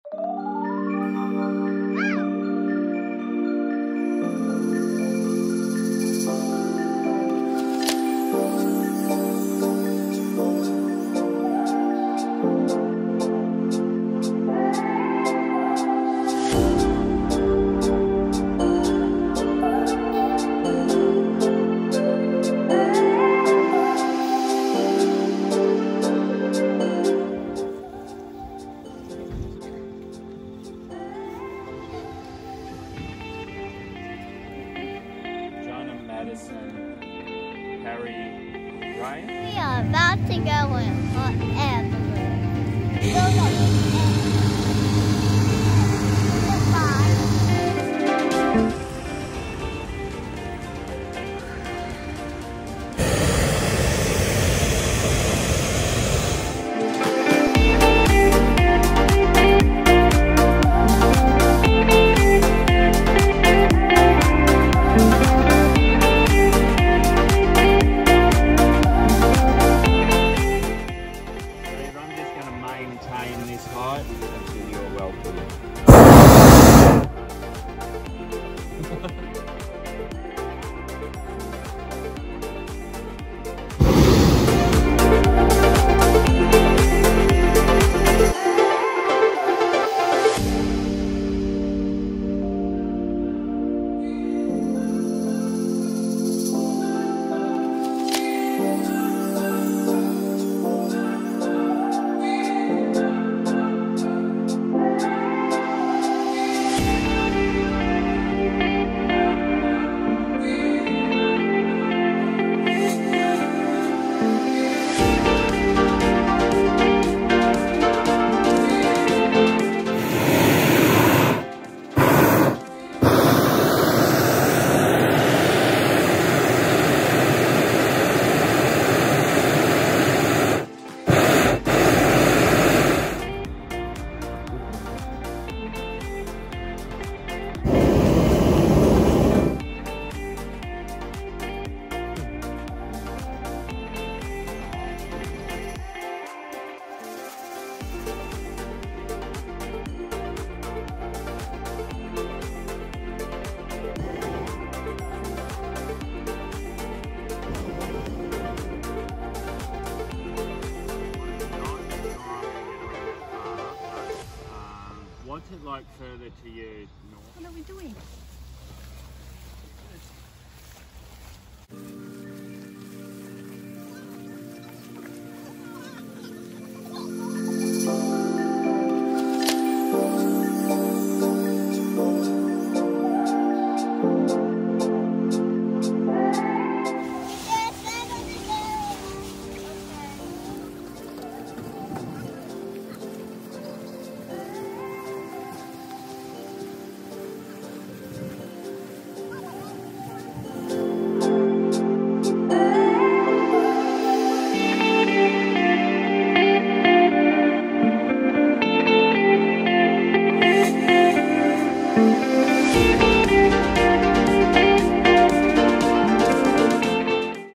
My Birthday Trip - Hot air balloon over Hunter Valley, NSW Australia
Our Hot Air Balloon Flight with winery breakfast, over Hunter Valley, NSW Australia back in April this year.